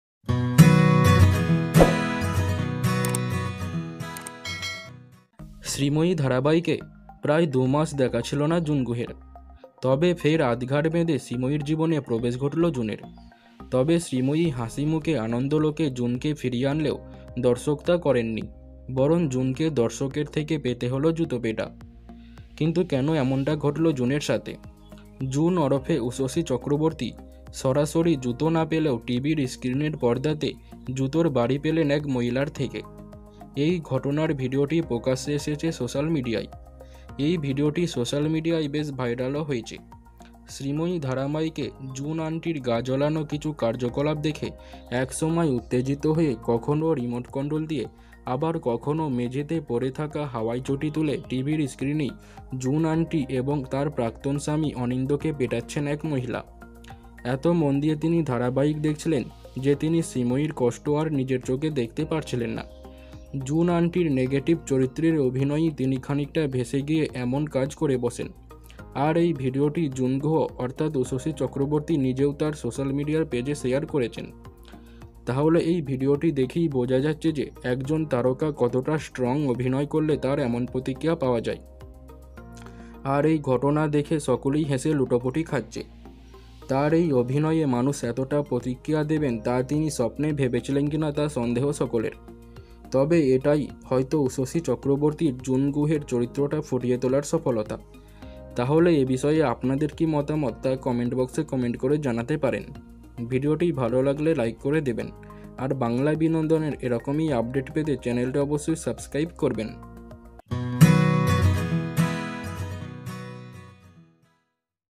জুন আন্টিকে জুতো মারলেন এক মহিলা কিন্তু কেন জানুন বিস্তারিত / Sreemoyee / Star jalsha
Sreemoyee serial news.